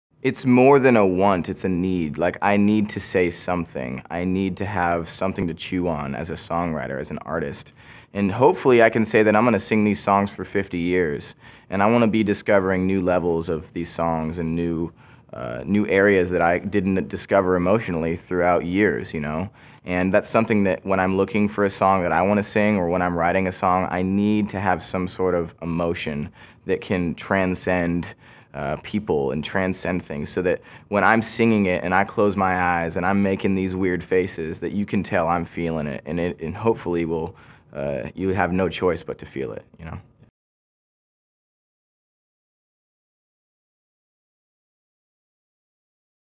How Is Devin Dawson Exploring Emotion In His Music?
Devin Dawson wants his music to stand the test of time, and believes that through exploring the emotion of his songs he will be able to connect with his audience on a brand new level.

Listen Free to More Artists Like Devin Dawson on Your Phone!